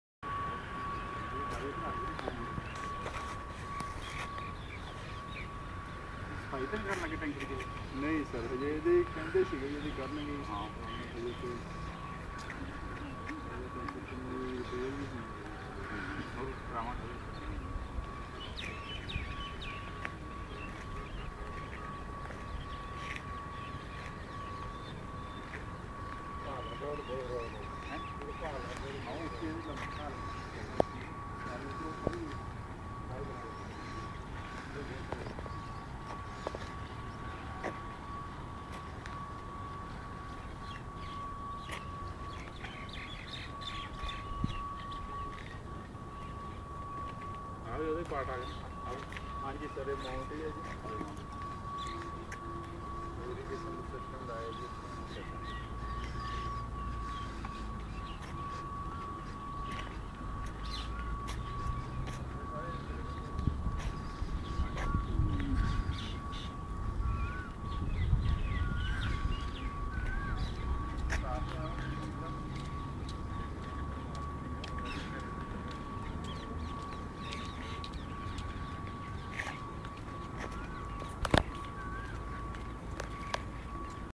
Up Coming Jogging Park in Bathinda....dream project of Punjab Chief Minister P.S.Badal...
A first of its kind in the malwa area a jogging park is under construction at Bathinda. The Chief Minister Punjab S Parkash Singh Badal is keen to make it best possible for the citizens of the Bathinda. Earlier he had in 1979 gifted a rose garden to Bathinda which is known to be one of the best garden in Punjab.